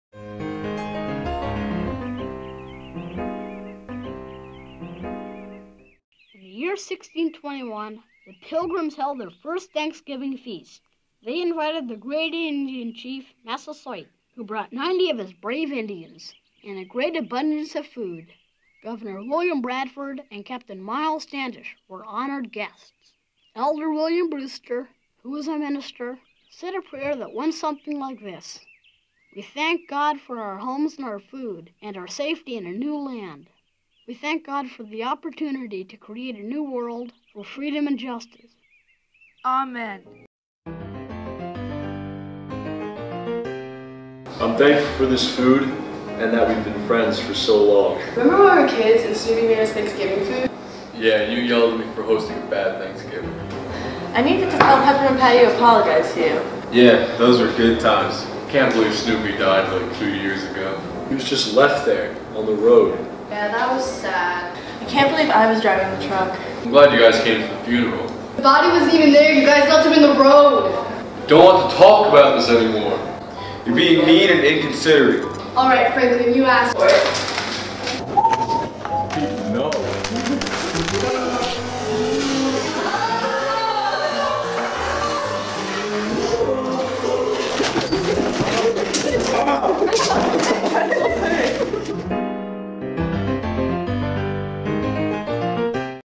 Modern Charlie Brown Thanksgiving
The Broadcasting 2 team wonders what Charlie Brown's Thanksgiving would look like 40 years later.